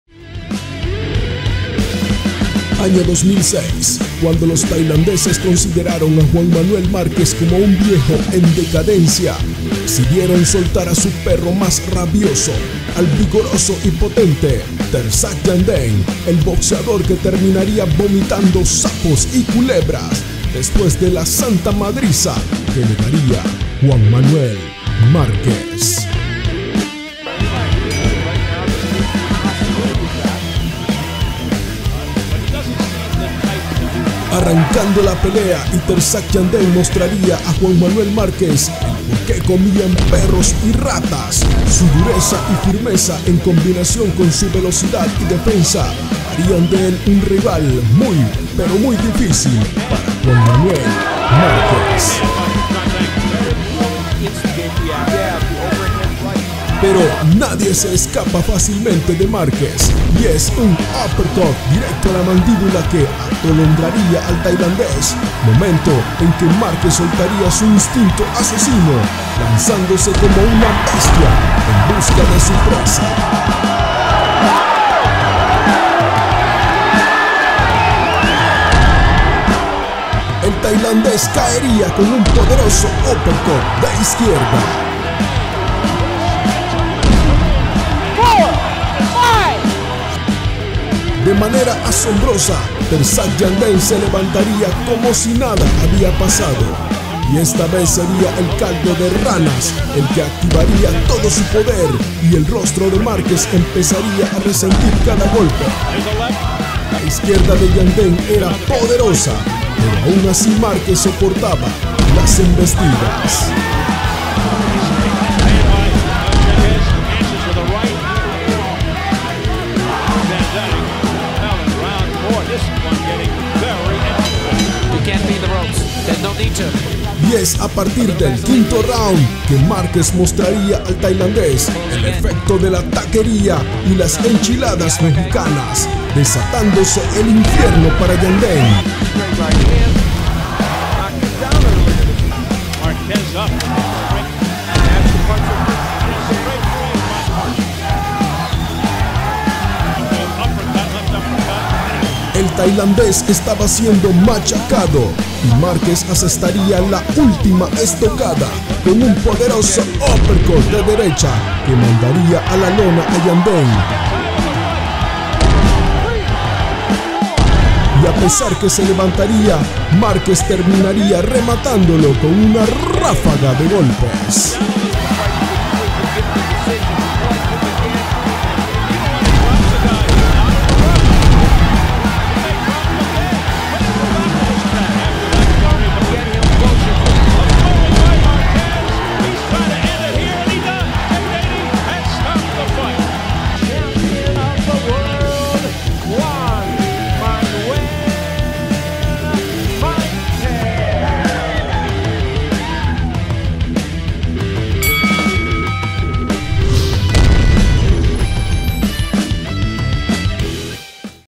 Llamó VIEJO "CHANGO" INSERVIBLE a Juan Manuel Marquez y Pagó muy CARO ! las consecuencias.
Cuando los Tailandeses sacaron a relucir a su gran figura boxística Terdsak Jandaeng pensaron que acabarían fácilmente con el mexicano Juan Manuel Márquez a quien consideraron débil y viejo sin imaginarse que aquella noche daría una de las mejores demostraciones de poder y gallardía arriba del ring, terminando el Jandaeng con la peor derrota propinada en toda su carrera boxística